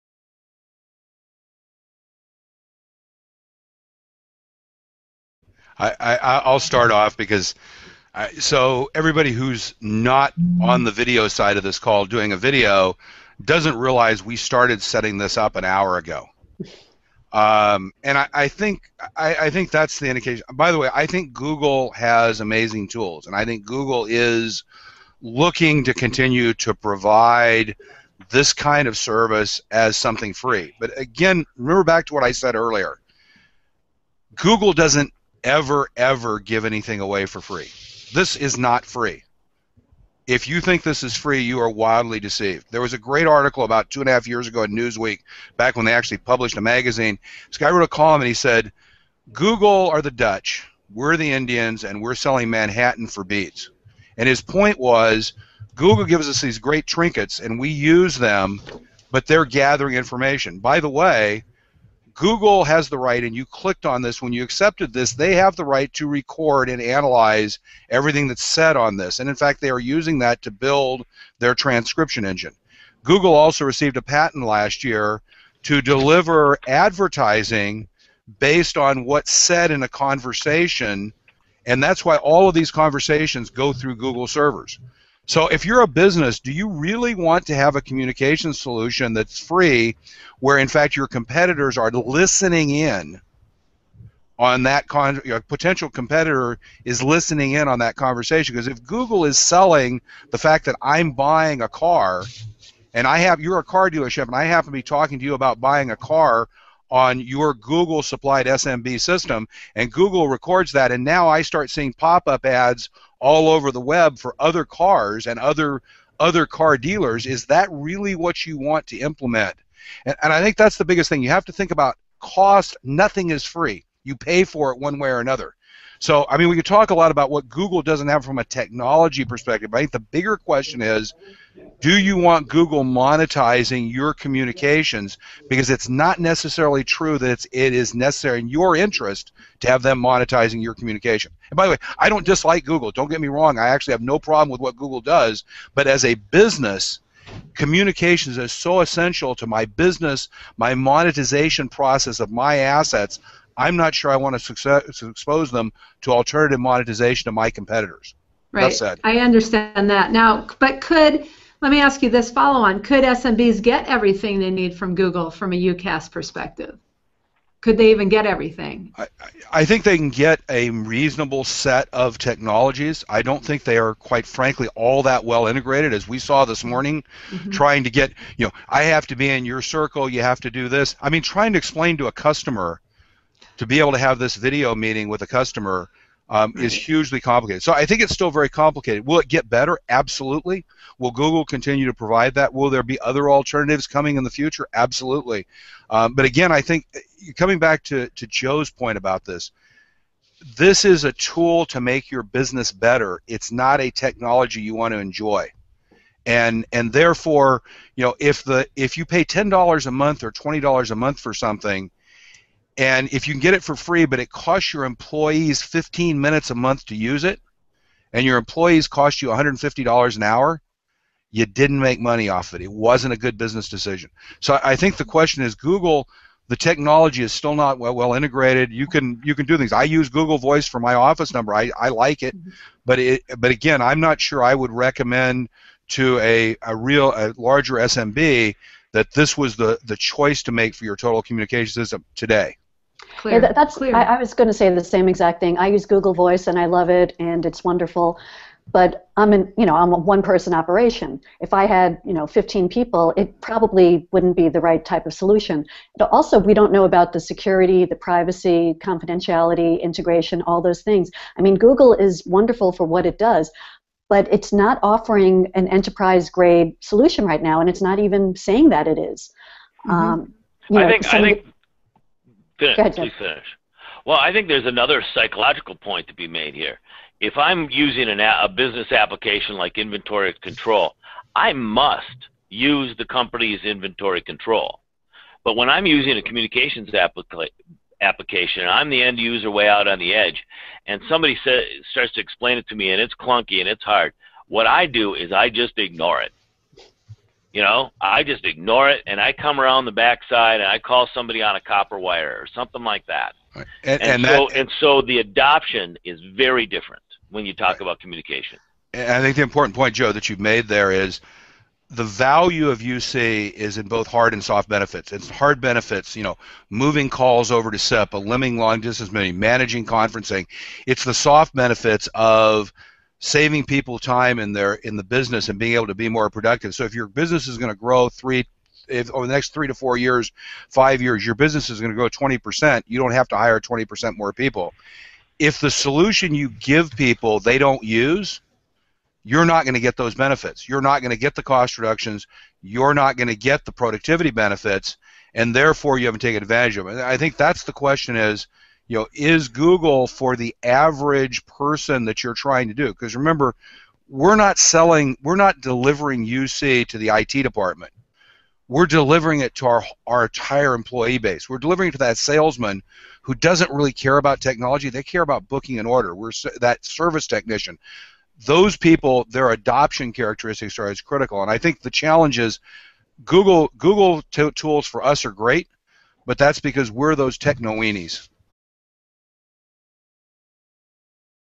Google vs. UCaaS: At What Point is Google Not Good Enough?
Unified Communications as a Service is becoming a popular way for SMBs to get the capabilities they need in a flexible, cost effective package.  Google has a set of applications that are similar to those of a UCaaS deployment but are Google apps robust enough to fill the UCaaS shoes?  UC Expert Phil Edholm from UCStrategies explains...